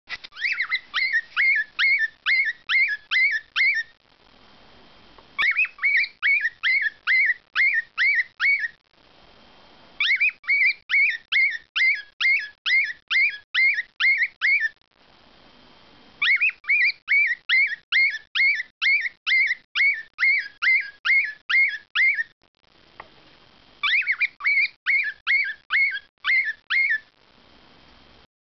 Syd singing toJake.30.8.11
My boyz and their noise.Sydney the dad in corner and Jake the son on front perch.